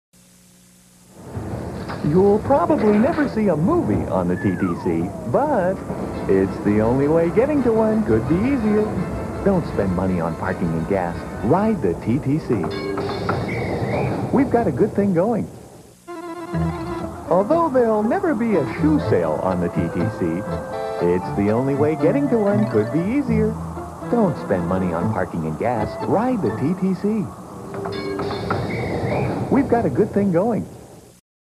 Toronto Transit Commission 1988
Join the TTC on ROBLOX!

A couple more of the short "We've Got A Good Thing Going" spots which were everywhere in 1988.